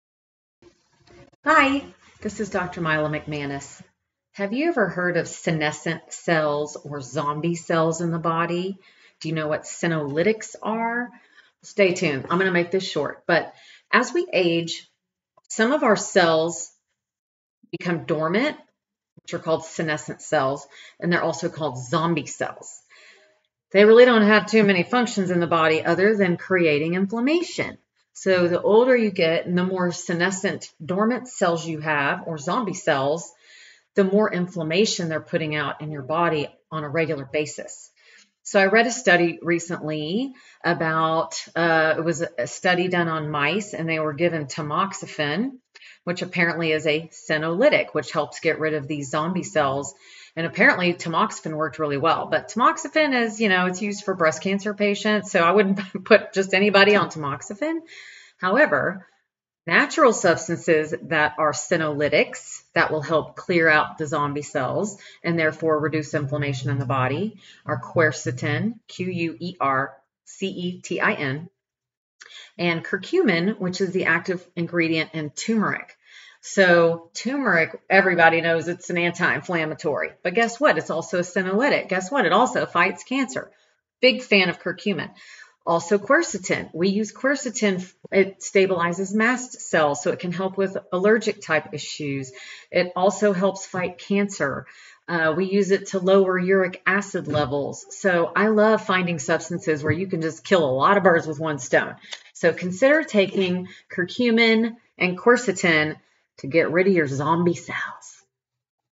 Dr McManus on Zombie Cells and Senolytics
What are Zombie Cells and what would a functional medicine doctor say to do about them? Dr McManus weighs in on what she prescribes to her patients.